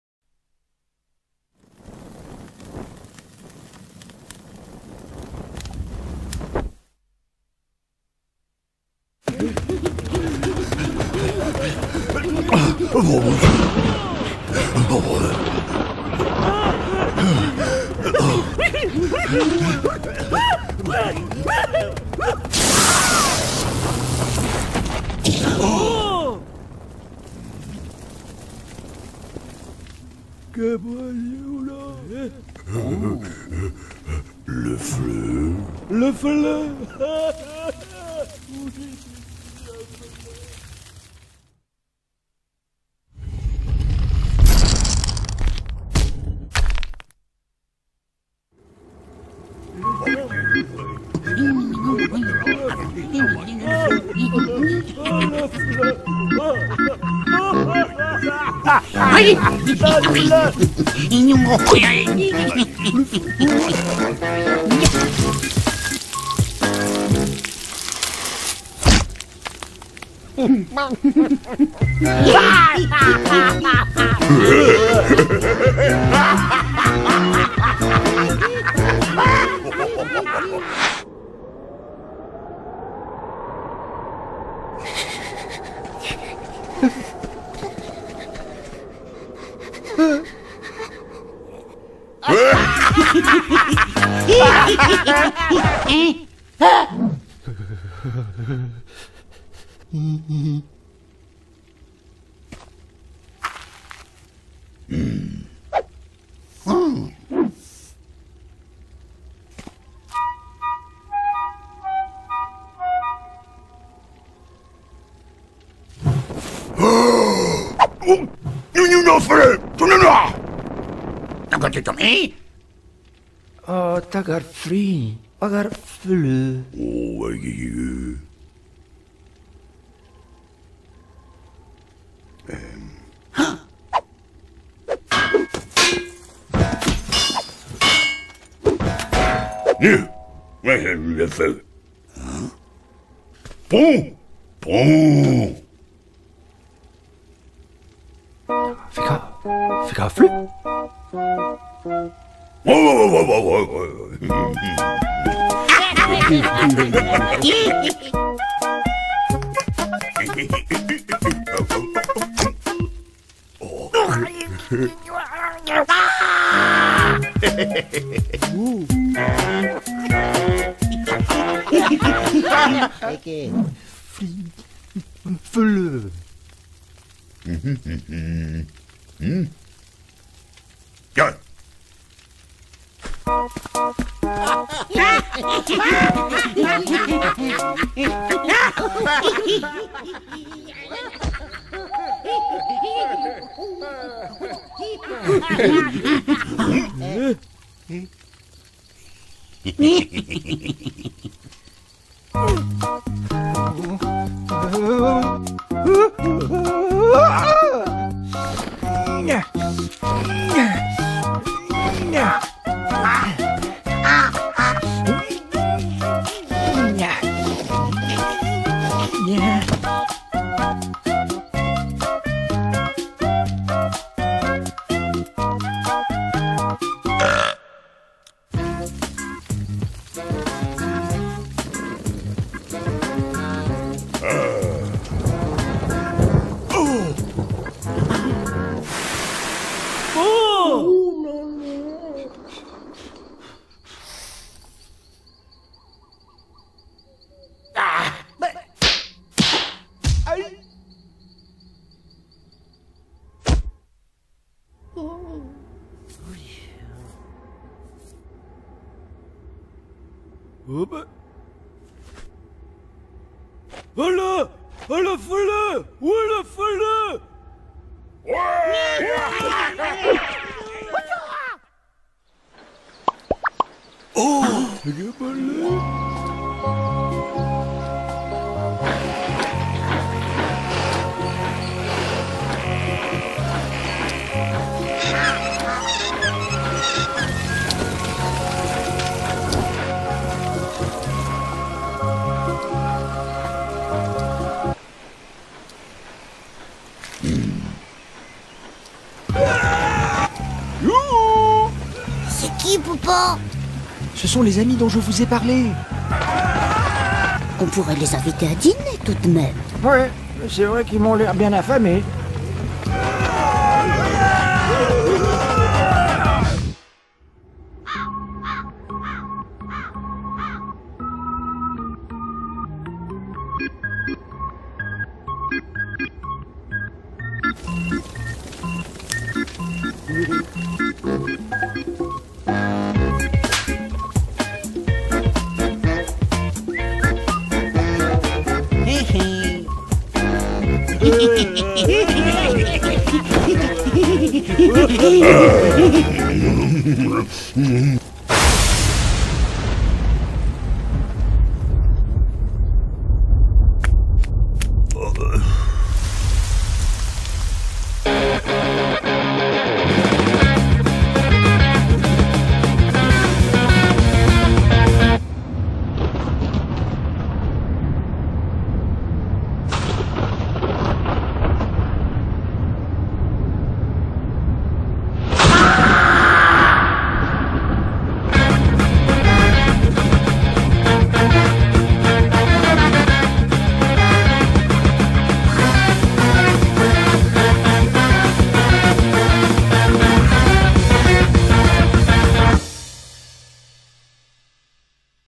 STONE AGE | ANIMATION CARTOON | SHORT FILM
веселая анимация, как в древние времена люди добывали огонь !! как бы его добыть)))
весёлый и позитивный мультфильм!

funny animation, as in ancient times, people made fire !! how to get it))) funny and positive cartoon!